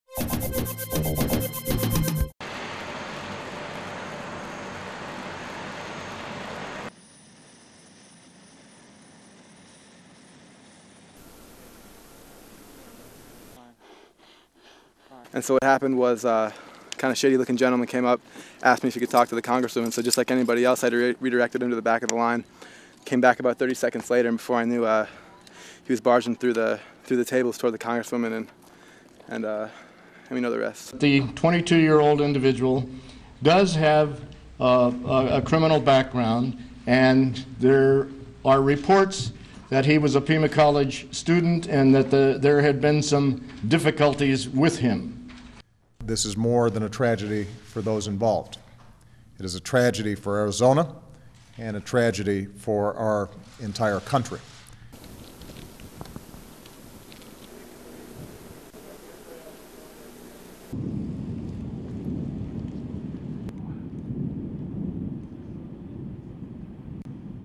US lawmaker gravely injured in shooting; 6 dead
A US lawmaker was in critical condition after being shot in the head Saturday by a gunman in Tucson, Arizona, in an attack which killed six including a federal judge and a nine-year-old girl. The gunman -- named by media as Jared Lee Loughner, 22 -- had a criminal past and was unstable but not insane, said Sheriff Clarence Dupnik, adding that the man was not talking to police officers in custody. Duration: 01:07.